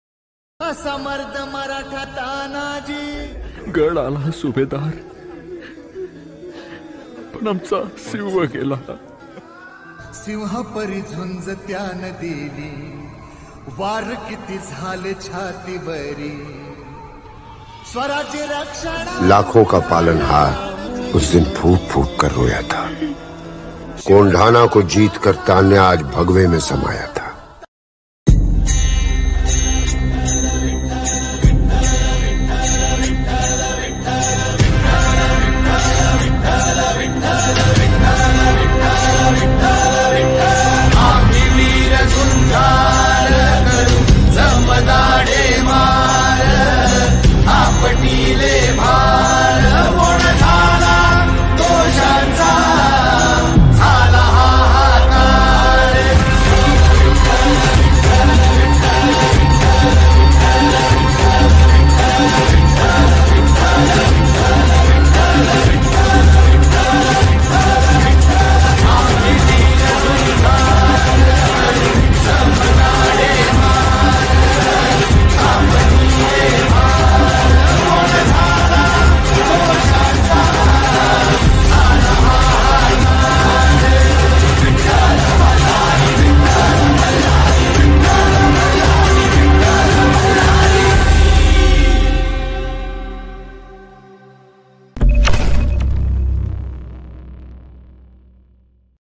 Tanaji Malusare Samadhi Renovation Bhumipujan
Witness the historic 🚩 🚩" Subhedar NarveerTanaji Malusare Samadhi Renewal Ceremony" 🚩 event, captured by SSK Creation at Umrath, Mahad, Maharashtra. Organized by the Maharashtra Government, this event honors the legacy of Tanaji Malusare, a revered warrior from Shivaji Maharaj's era.

Watch the full event coverage, including live streaming, cinematic gimbal shots, online video mixing, and stunning live drone footage. Key figures like MP Sunil Tatkare and Maharashtra Ministers Uday Samant and Bharat Gogavale attended the event.

Location:- Umrath, Mahad, Maharashtra

🎥✨ Capture Your Moments with Perfection!
For Events Videography & Photography, Sound Systems, and Lighting Solutions